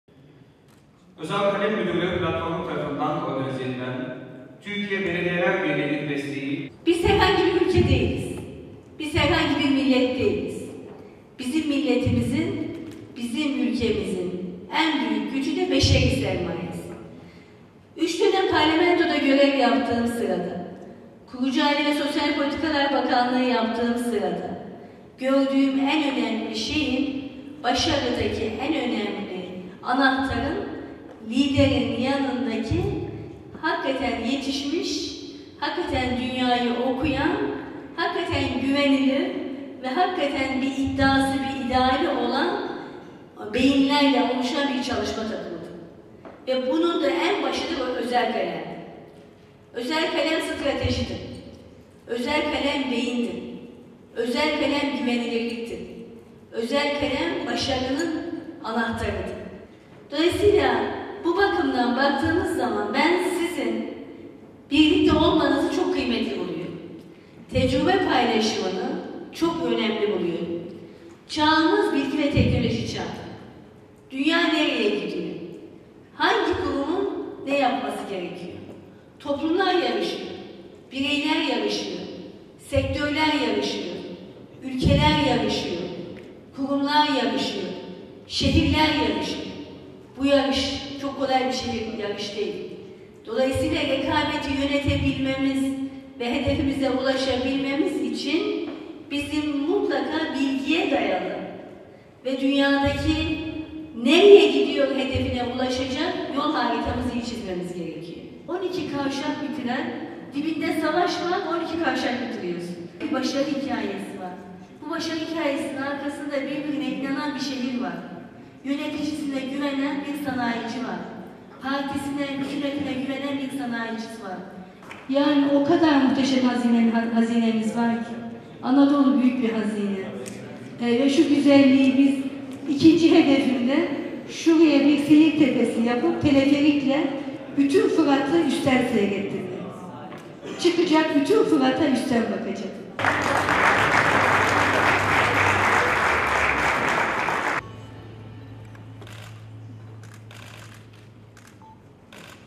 "Zoru başarabilmenin yolu kardeş olmaktan geçiyor"
Gaziantep Büyükşehir Belediye Başkanı Fatma Şahin, zoru başarabilmenin yolunun bir, beraber ve kardeş olmaktan geçtiğini söyledi.